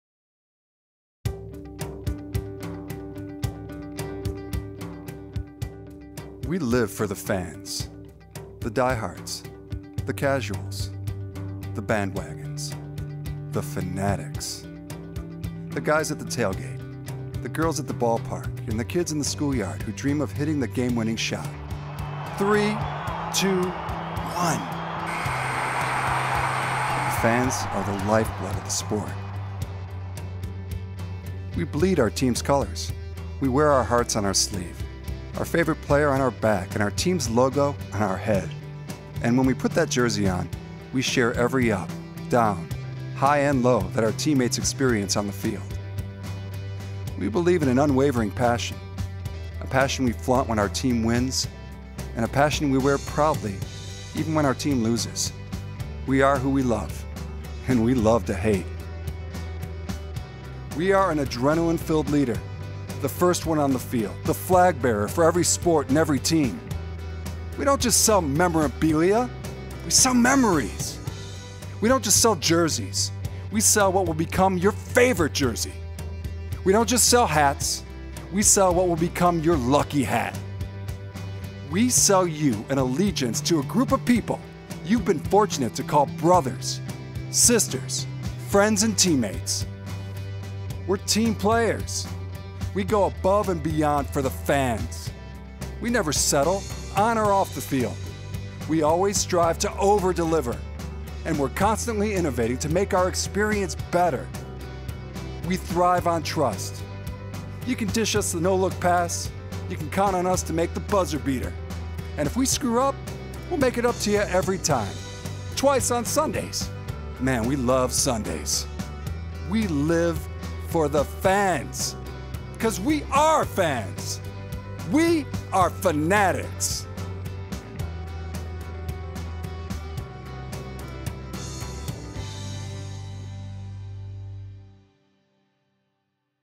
Motivational Corporate Presentation
Created to motivate the employees and clients of a large online retailer. Combines strong corporate messaging with actual employees in the workplace to promote the importance of the work everyone does.